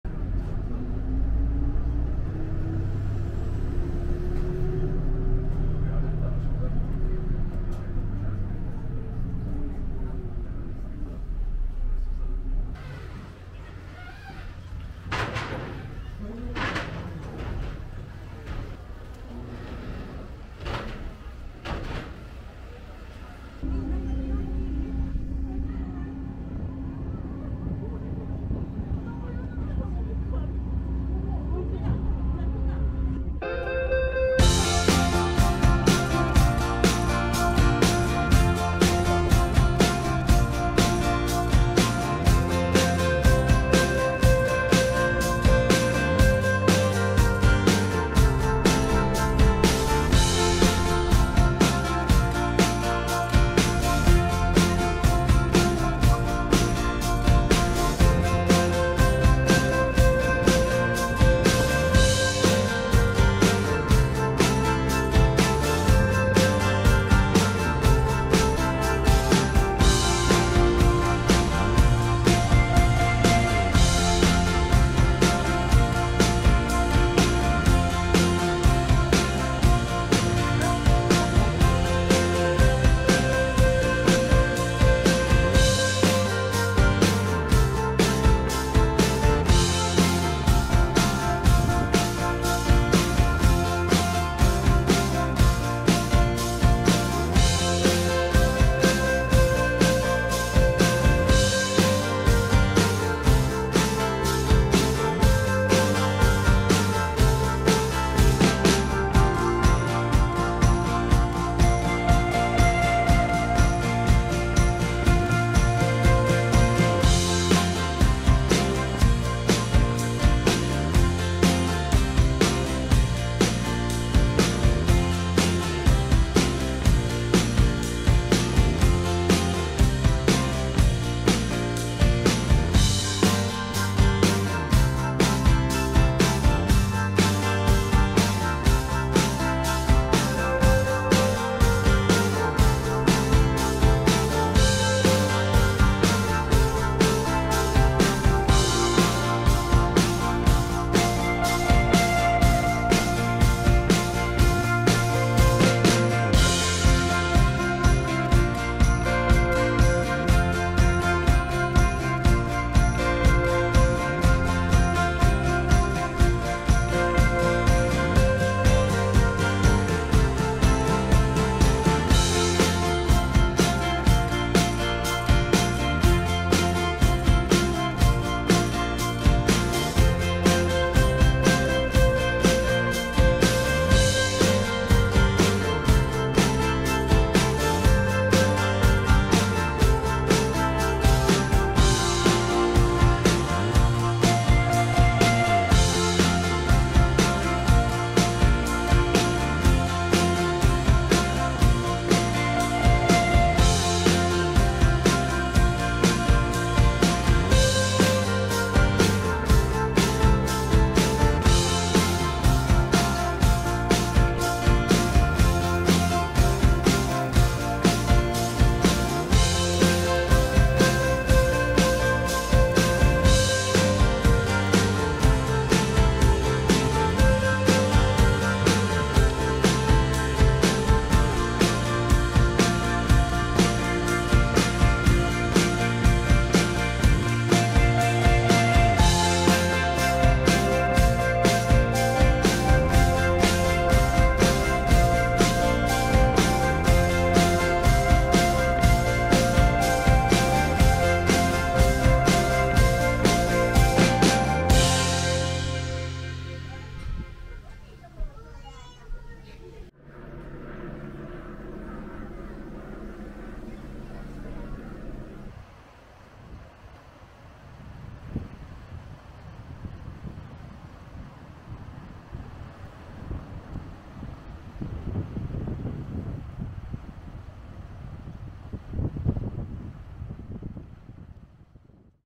Nokonoshima Island Park 🏝️ Flower Paradise, Perfect Day Trip From Fukuoka City
Nokonoshima Island is just 10 minutes by ferry from the Meinohama Terminal in Fukuoka City. Nokonoshima Island Park is located on the northern tip of the island. This park is filled with blooming flowers throughout all four seasons. It's a perfect place to enjoy picnics and barbeques. Also, it's a quick retreat into nature from the nearby city.